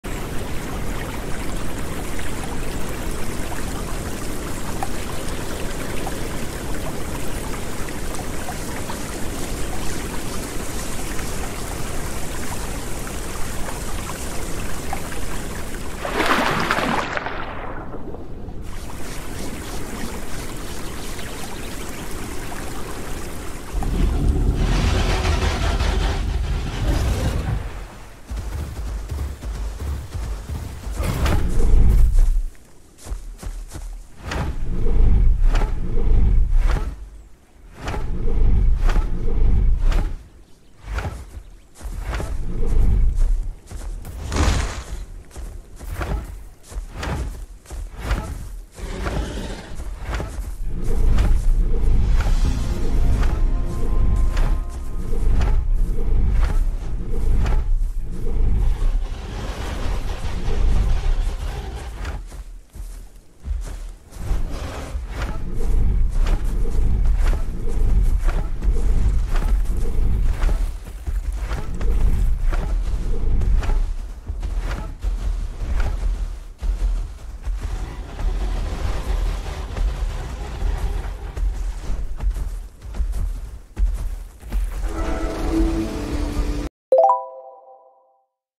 Sarco fricks around and fights an Argent
Decided to frick around, and found out XD
Got them to slightly below half.